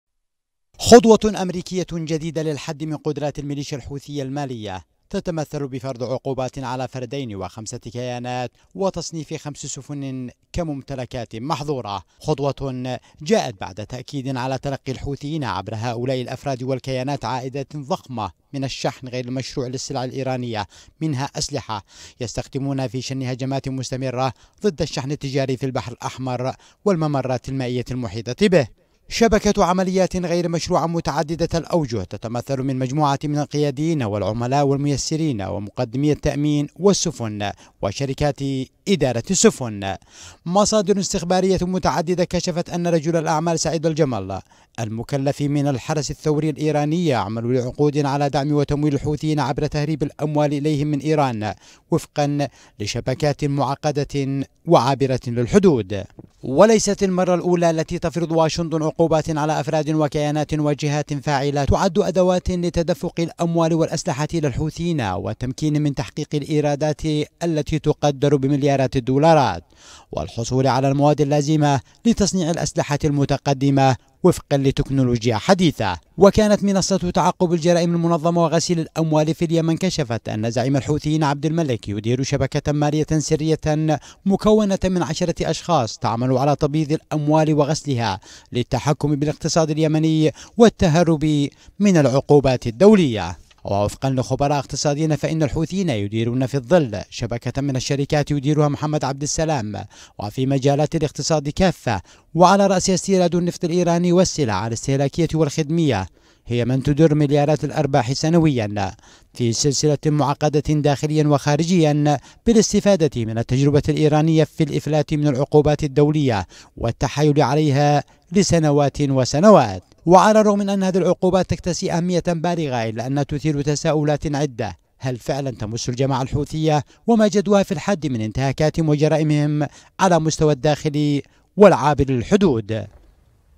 تداعيات العقوبات الأمريكية على أفراد وكيانات حوثية تتلقى دعماً إيرانياً - تقرير: أمين العتيبة
فرضت الولايات المتحدة الأمريكية عقوبات على فردين وخمسة كيانات حوثية وتصنيف خمس سفن كممتلكات محظورة، حيث يتلقى الحوثيون عبر هؤلاء الأفراد والكيانات عائدات ضخمة من الشحن غير المشروع للسلع الإيرانية في مقدمتها الأسلحة.